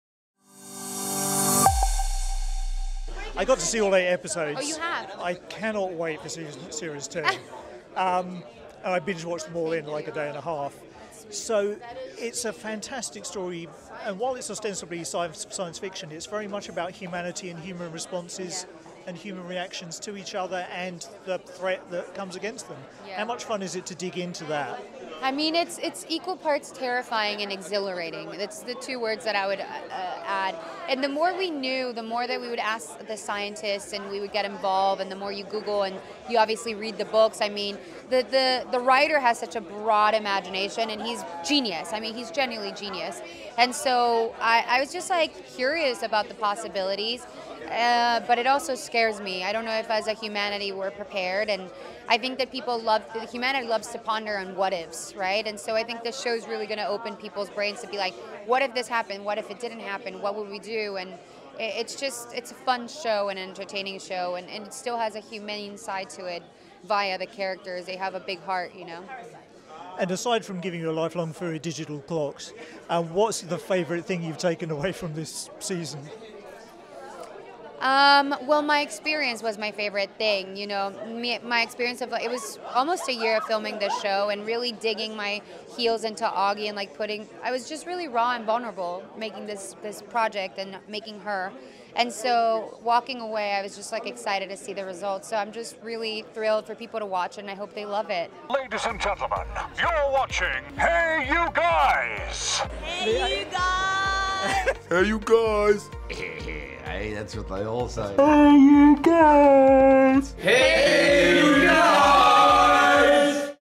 Eiza González - 3 Body Problem UK Premiere Red Carpet Interview
Eiza Gonzalez is interviewed at the UK Premiere of Netflix's 3 Body Problem,  the genre-bending mystery series from Game of Thrones creators David Benioff, D.B. Weiss and Alexander Woo. The series stars Jovan Adepo, John Bradley, Rosalind Chao, Liam Cunningham, Eiza González, Jess Hong, Marlo Kelly, Alex Sharp, Sea Shimooka, Zine Tseng, Saamer Usmani, Benedict Wong and Jonathan Pryce.

Colin Hart and Ethan Hart were on the red carpet.

Plot: A young woman’s fateful decision in 1960s China reverberates across space and time into the present day. When the laws of nature inexplicably unravel before their eyes, a close-knit group of brilliant scientists join forces with an unorthodox detective to confront the greatest threat in humanity’s history.

The show will launch on the streamer from the 21st of March.